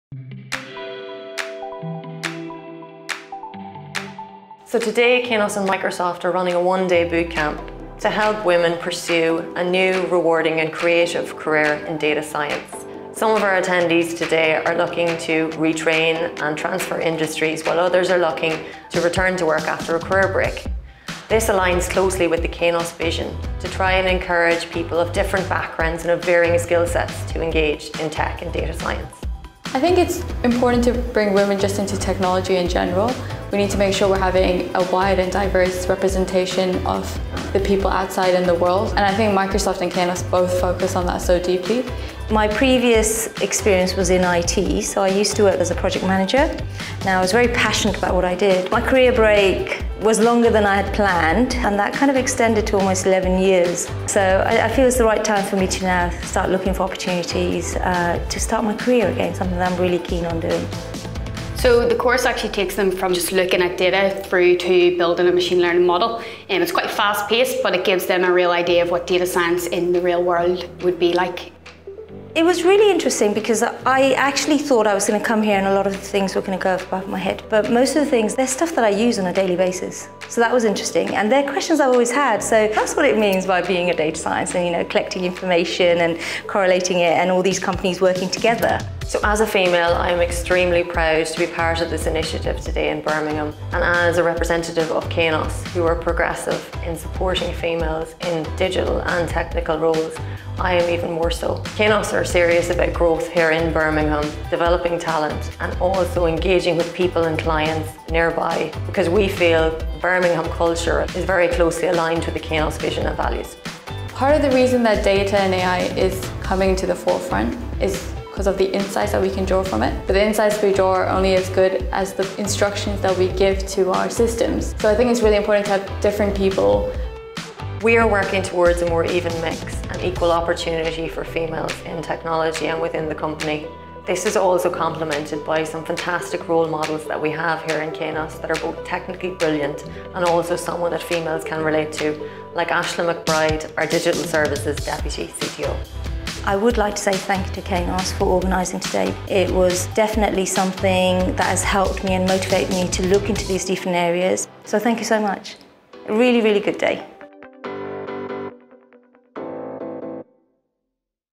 Kainos & Microsoft: Bringing more women into Data Science
Kainos, Microsoft’s 2018 UK Partner of the Year, hosted a one-day bootcamp to help women of all backgrounds to pursue a new, rewarding and creative career in Data Science.